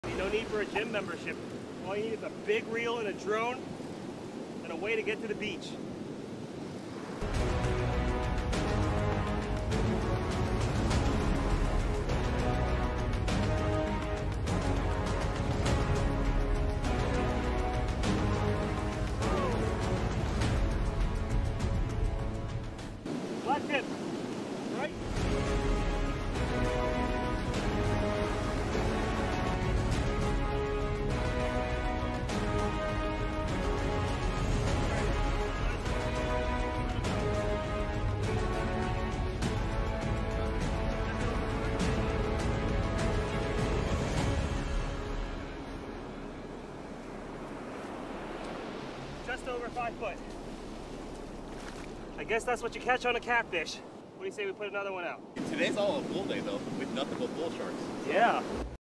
Catching a Bull Shark from the Beach!
Big dangerous fish landed at the beach! (Safely released)